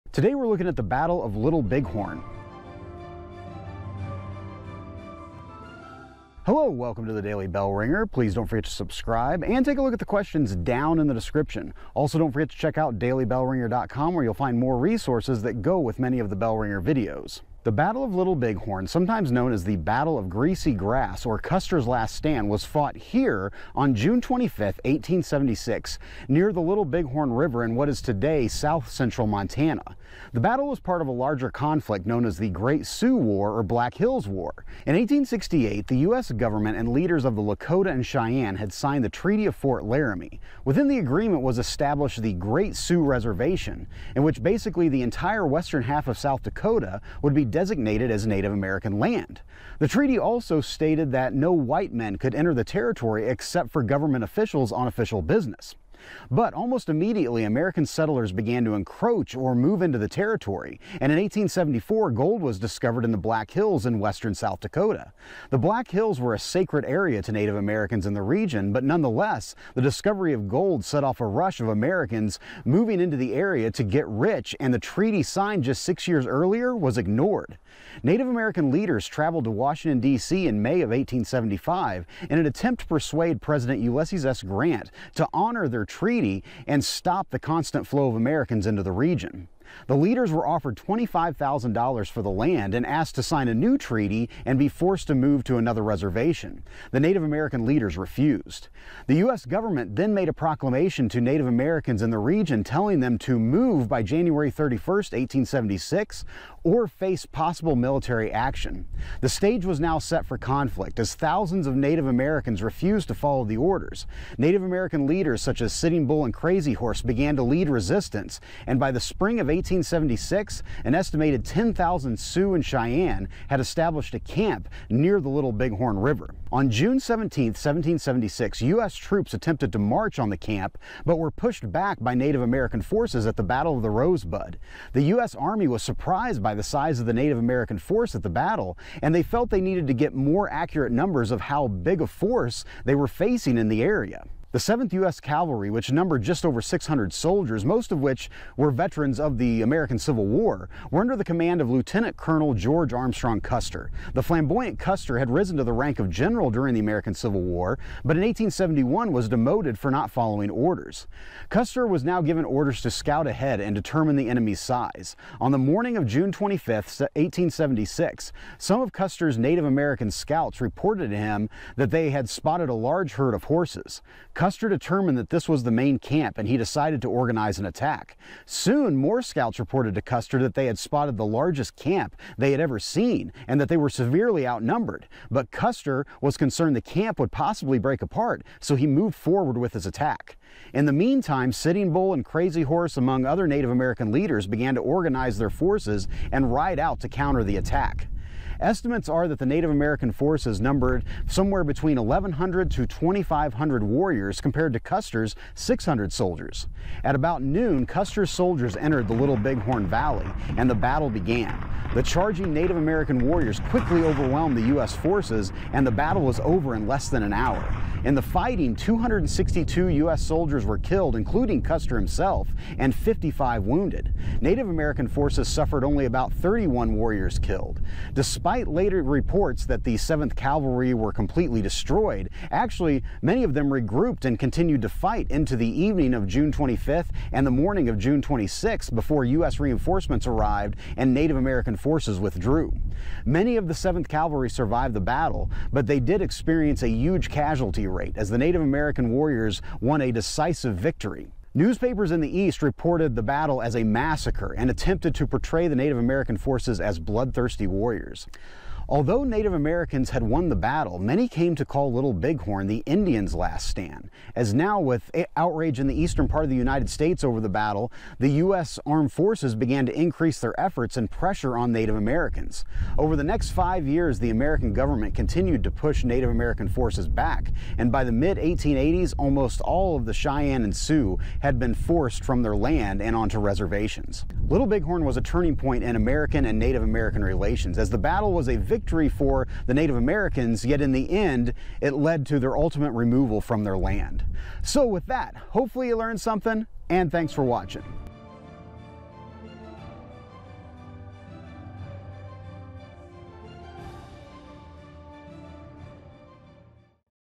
Battle of Little Bighorn | Daily Bellringer
Battle of Little Bighorn: By the mid-1870s tensions between Native Americans and Americans moving onto Native American land had reached a breaking point. The two sides would meet near the Little Bighorn River in what would be one of the most famous battles of the Wild West Era. Questions below:

1.  In what present-day state did the Battle of Little Bighorn take place?
2.  Where was gold discovered in 1874?
3.  Approximately how many Native American Warriors were in the battle?
4.  TRUE OR FALSE: The entire 7th Cavalry was destroyed in the battle.
5.  Why do you think the U.S. government and Native Americans could not find a peaceful resolution to settlers moving west?

ADDITIONAL MATERIALS AND WORKSHEETS FOR MANY OF THE BELLRINGER VIDEOS